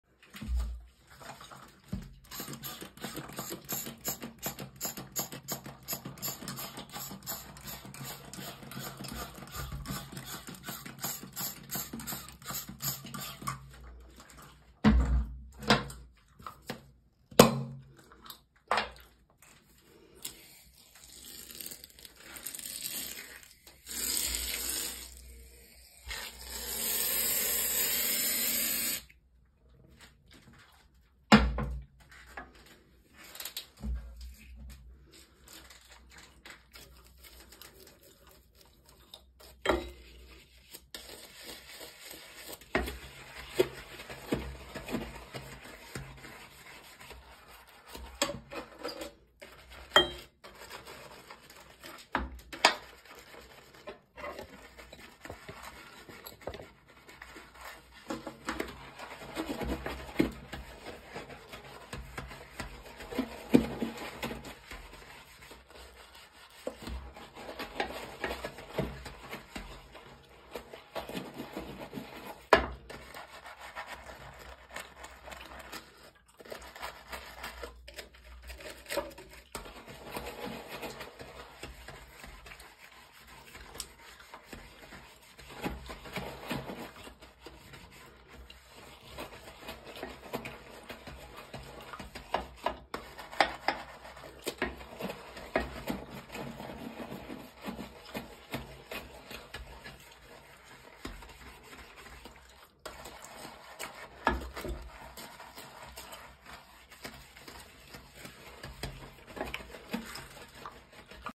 ASMR Sink Scrubbing - Foamy Sudsy Cleaning with a Brush - LOFI & NO TALKING
This is a sink scrubbing asmr video. I am cleaning my sink with limescale remover and toilet foam and using a brush. I love suds and soapy sudsy cleaning. It feels relaxed and fresh. Hope you like this lofi and no talking asmr video and get relaxed.